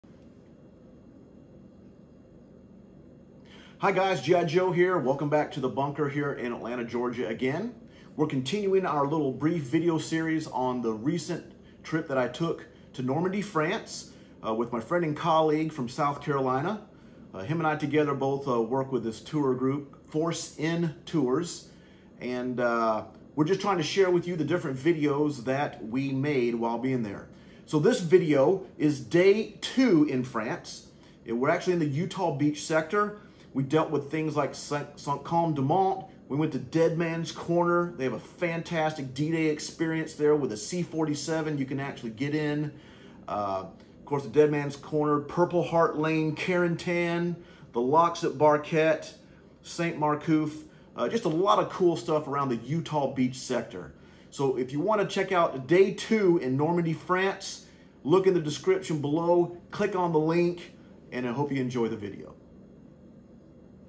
Normandy Tour Day 2 Recap!
Join GiJoe and Force N Tours for a Day 2 recap of their Normandy trip.